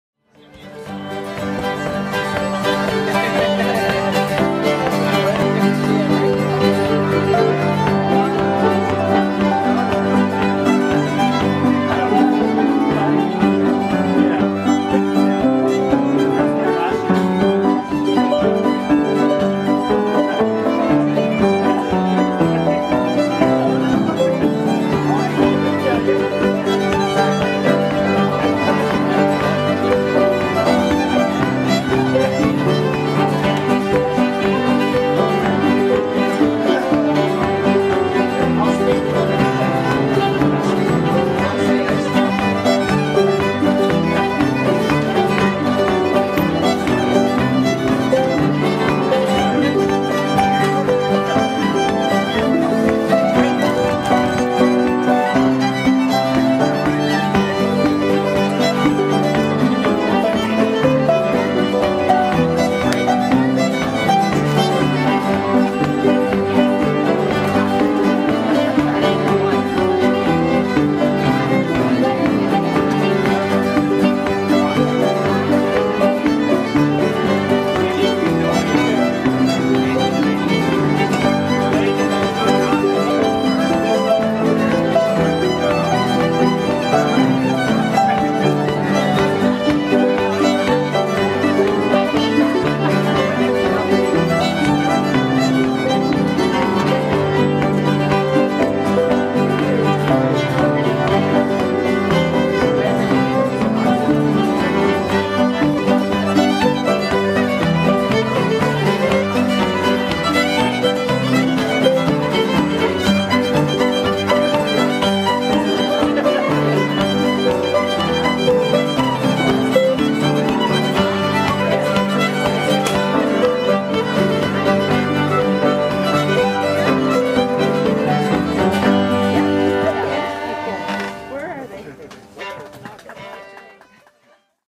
A Webster harp at Pembroke
Here's a Webster harp in a jam in "The Back Yard" at the Fiddle Festival in Pembroke, 20101